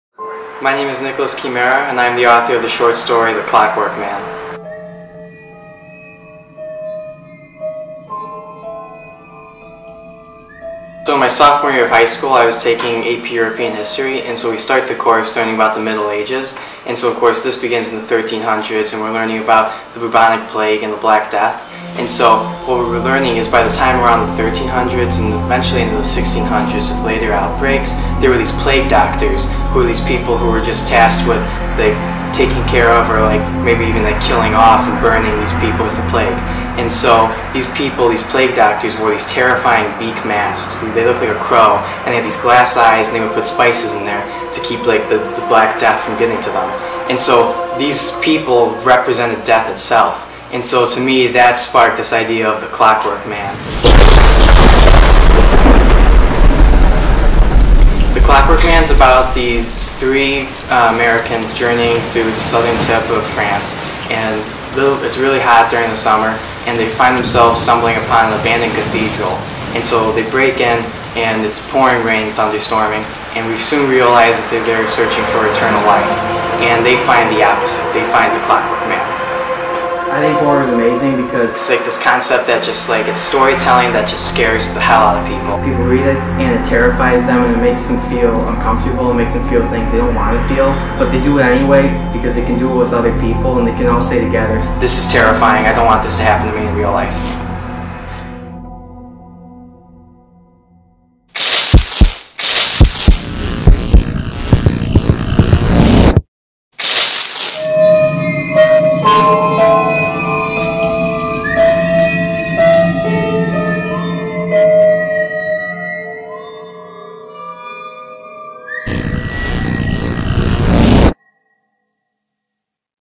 "The Clockwork Man", a short story by Nicholas Chimera
Nicholas Chimera is the prodigy author of "The Clockwork Man", a short story in the new horror graphic novel anthology "Cellar Door" from Chimera's Comics. You can check it out on Kickstarter

In "The Clockwork Man", 3 travelers face death in an abandoned cathedral while searching for eternal life.

"Cellar Door" is a 140 page scifi horror graphic novel featuring 14 stories cut from the same cloth as H.P. Lovecraft and The Twilight Zone